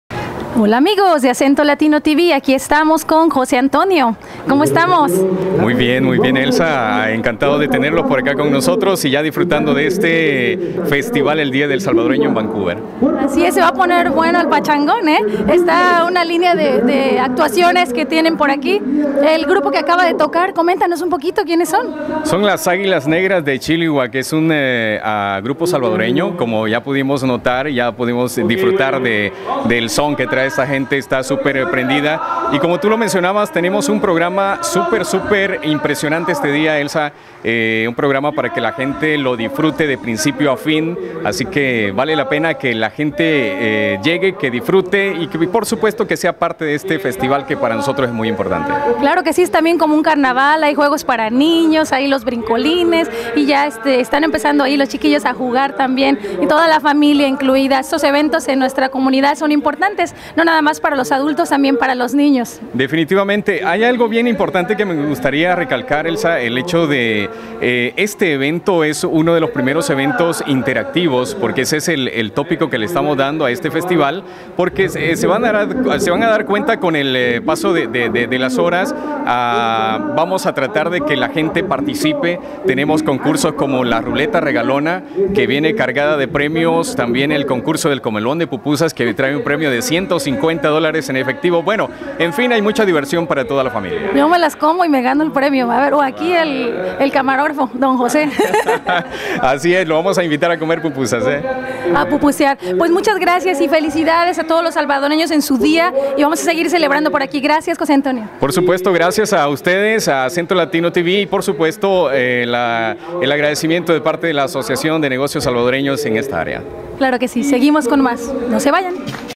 Entrevista a José Antonio
Elsa Rojas entrevista a José Antonio del comité organizador del Día del Salvadoreño